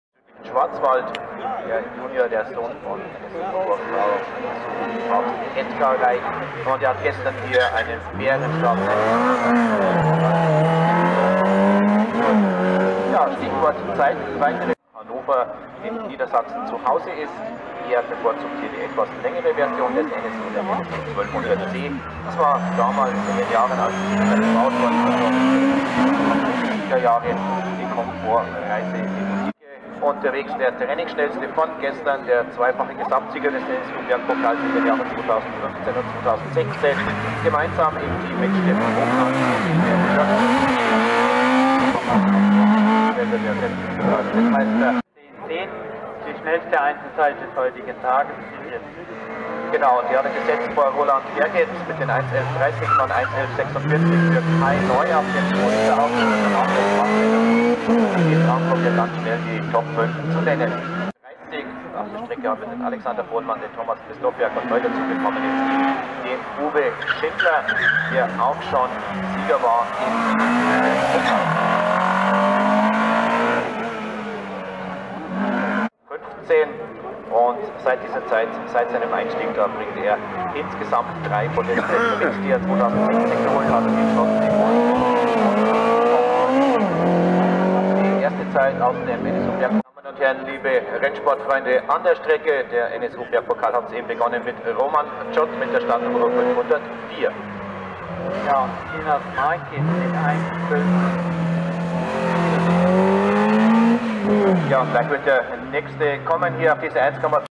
KW Berg Cup NSU Bergpokal beim Bergrennen Wolsfeld 2017
Die NSU in der Bitkurve.